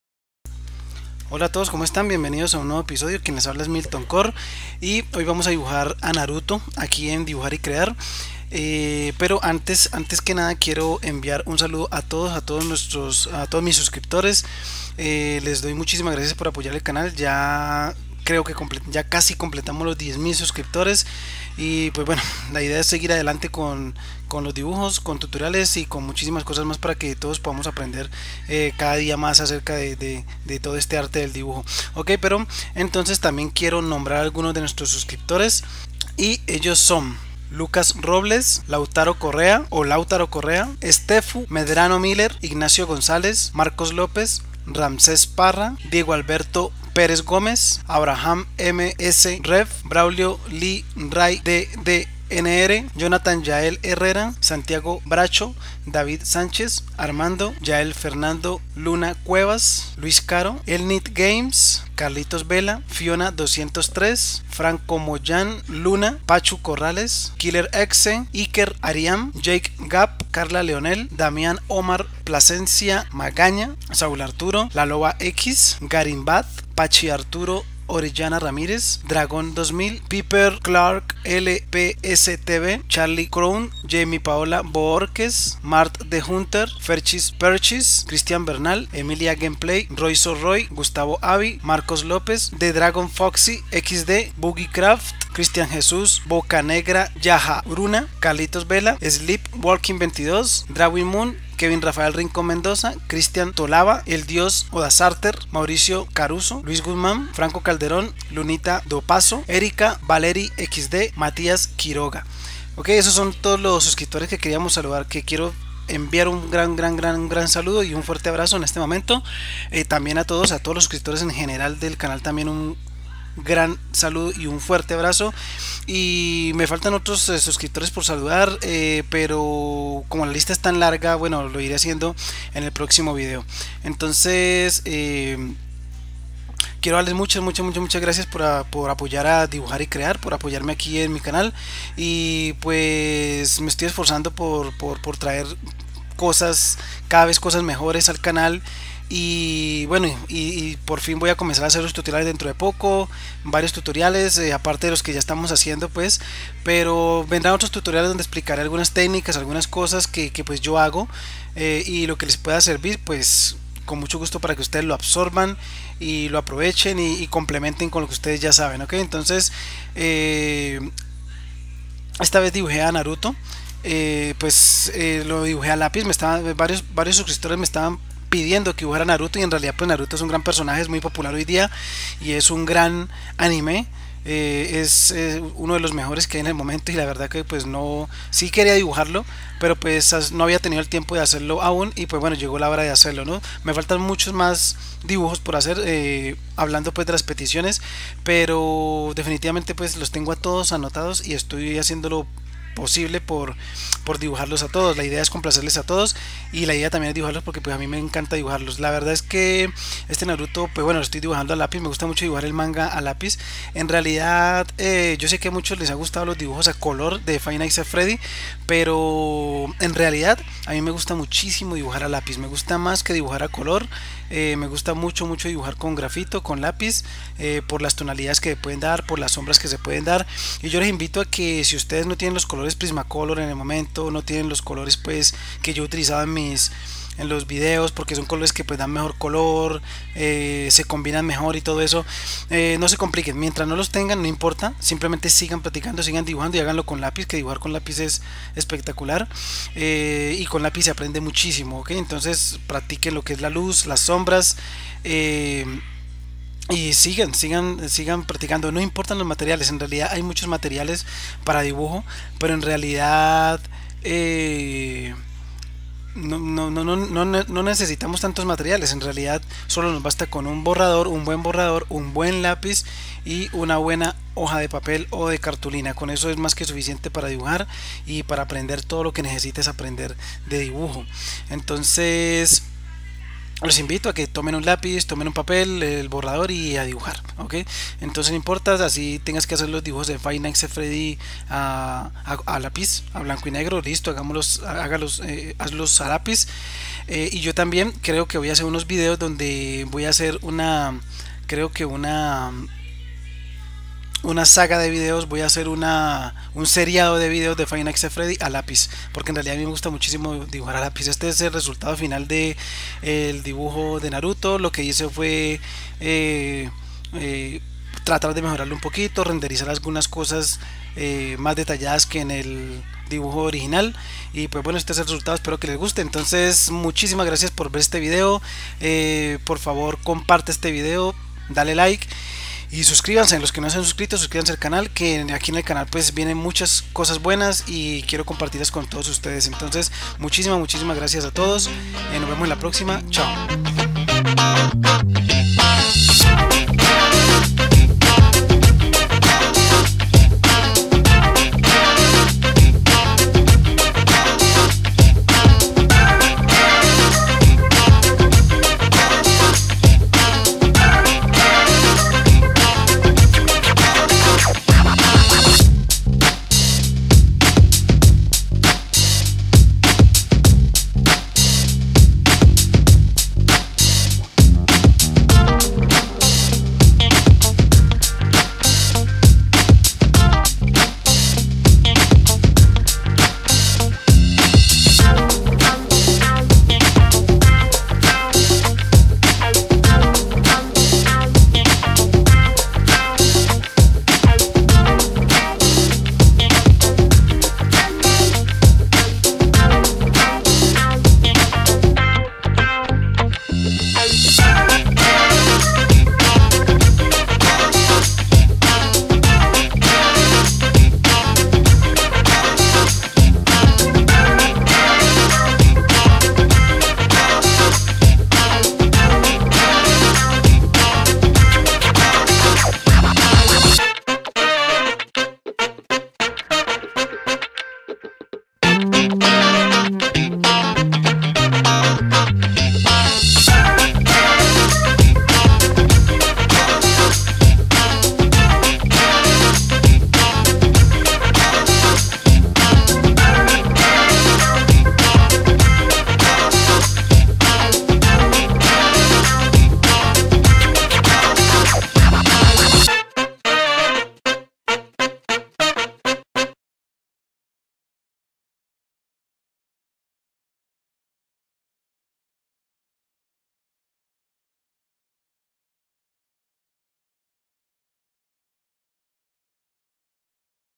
Dibujando a Naruto Con Lapiz | Anime
Hola. Dibujando a Naruto de uno de los anime mas populares. Varios suscriptores me lo pidieron y por fin esta el render de este personaje. Disfrute dibujándolo y creo que lo dibujare de nuevo pronto. Espero te guste y por favor compártelo y deja tus comentarios. Mil gracias!

MATERIALES:
Lapiz Steadtler 2B
Goma de borrar
Papel cartulina opalina